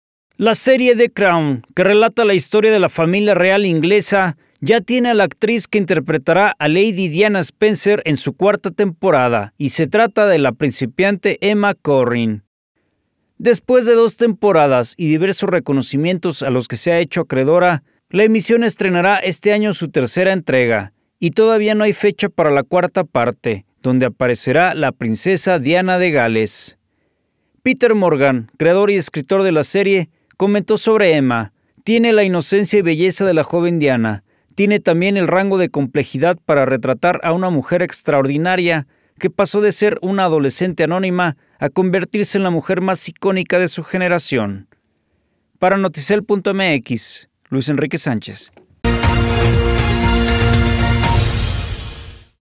Será Lady Di
La novata Emma Corrin dará vida a Diana Spencer en la cuarta temporada de "The Crown"